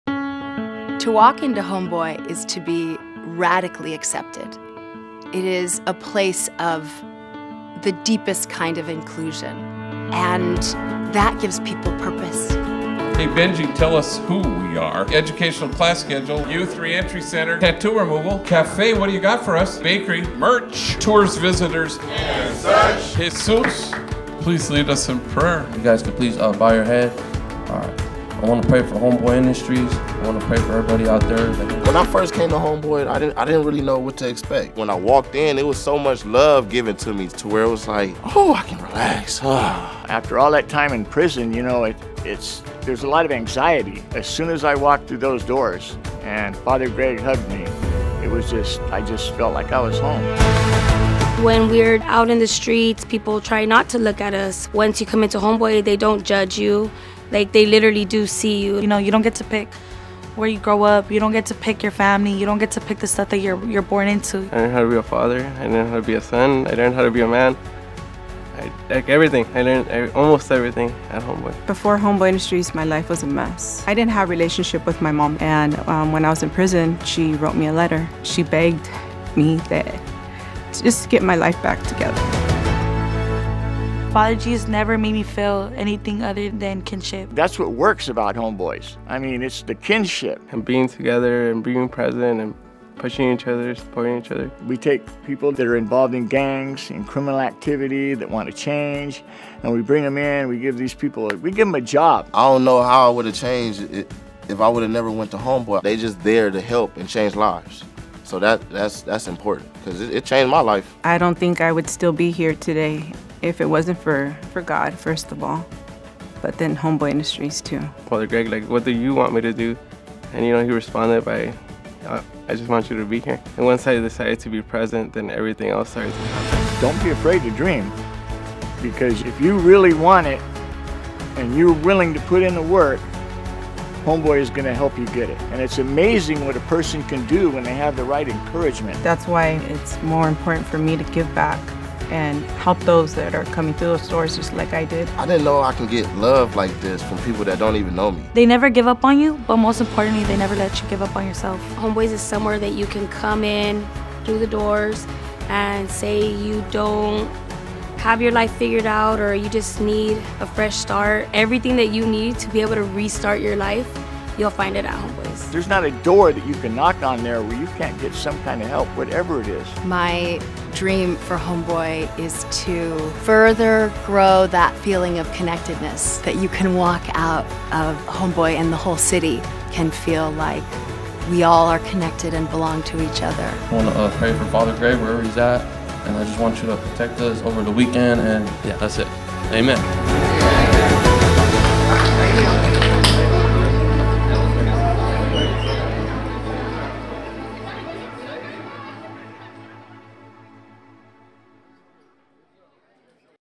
Homeboy Industries 2023 | A Community of Kinship: "We are all connected and we belong to each other"
Located in downtown Los Angeles, Homeboy Industries provides hope, training, and support to formerly gang-involved and previously incarcerated people, allowing them to redirect their lives and become contributing members of our community. By utilizing a combination of donations and social enterprises, Homeboy Industries is a 501(c)3 nonprofit organization whose innovative model of therapeutic wrap-around services has helped the organization become one of the most successful re-entry programs in the United States.